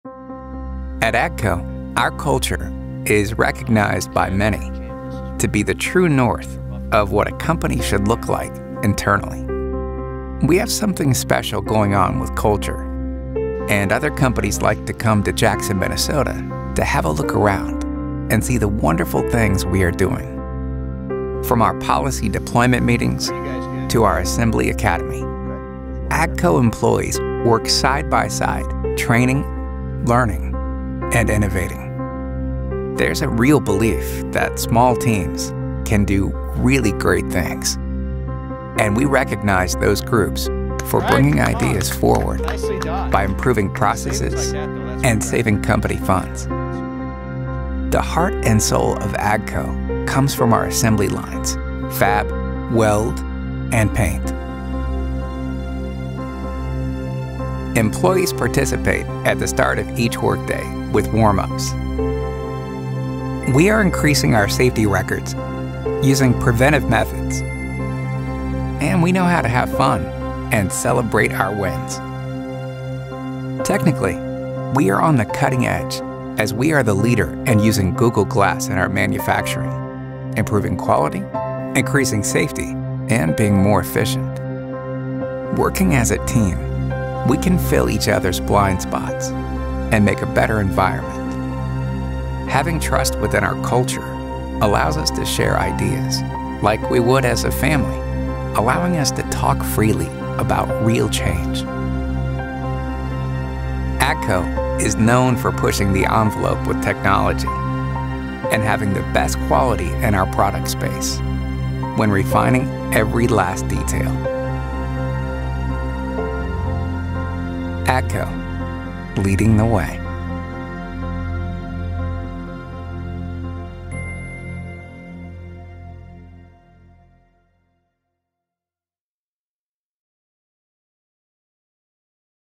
AGCO's Award-Winning Jackson, Minnesota Operations Leads in Culture & Technology
AGCO’s Jackson, Minnesota operations was named the 2017 Assembly Plant of the Year presented by ASSEMBLY magazine and received the 2017 AME Excellence Award presented by the Association for Manufacturing Excellence (AME). The internal culture within the plant and cutting-edge technology such as Glass assisted-reality wearable devices contributed significantly to this and AGCO's Jackson operations becoming a world-class manufacturing center. Learn more about these two awards:

AGCO’s culture is a model that other manufacturing companies aspire to follow. Employees work and thrive in Jackson. From our policy deployment meetings to the AGCO Assembly Academy, AGCO employees work side by side training, learning and innovating. Open dialog is encouraged, and employees are empowered to actively offer suggestions to help improve processes and save money.

This culture and the commitment of the entire Jackson team has allowed AGCO to be on the cutting-edge of technology and truly lead the way in manufacturing quality farm equipment.